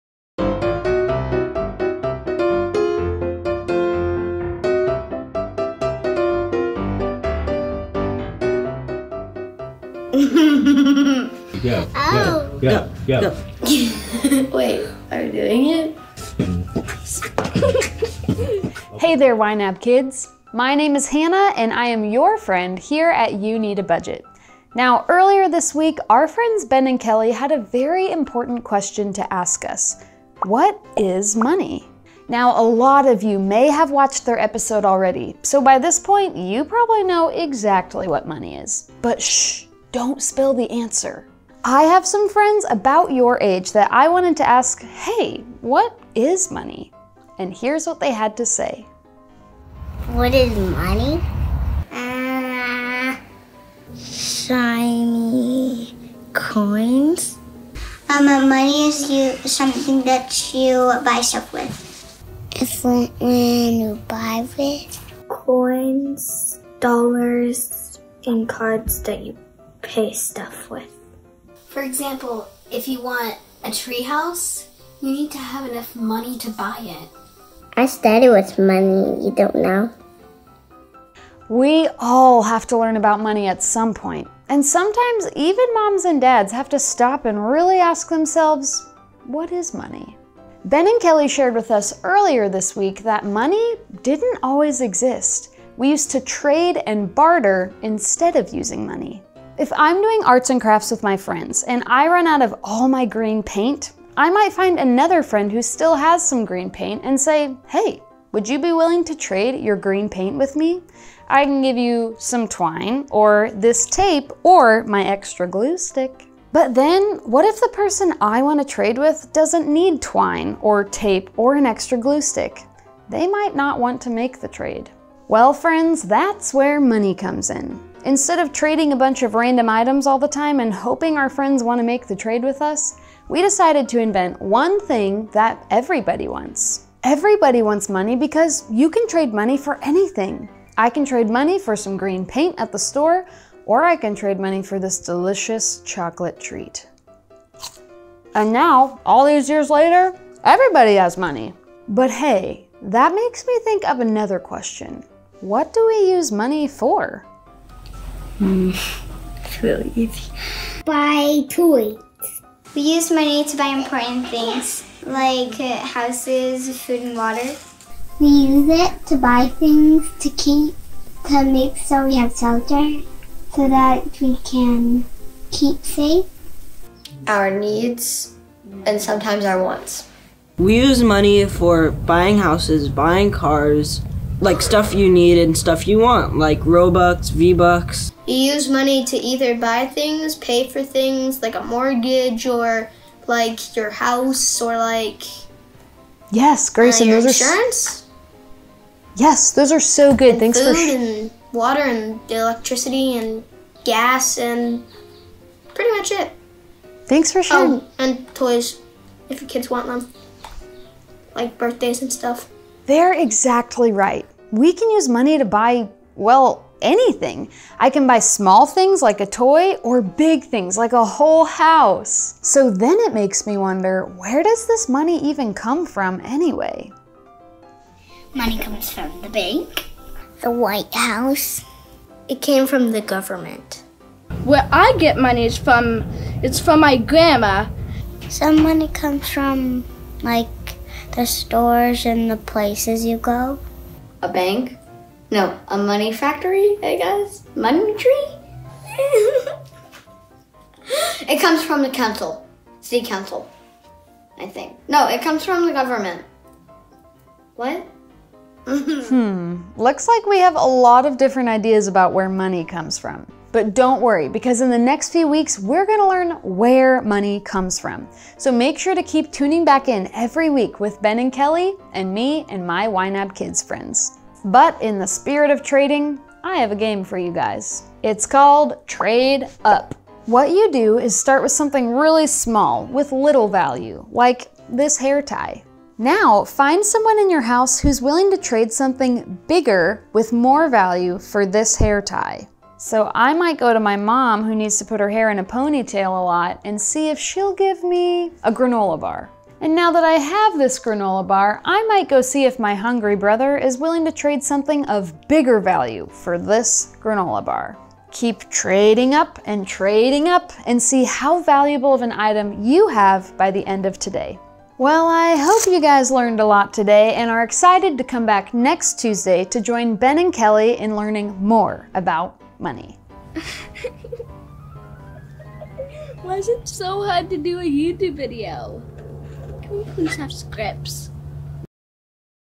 What is Money? | Kid Talk Edition
Earlier this week, Ben and Kelly taught us a little bit about the history of money: why we have it and what we did before we had money. We asked some YNAB Kids what they know about money, and share a game with you to practicing your trading skills!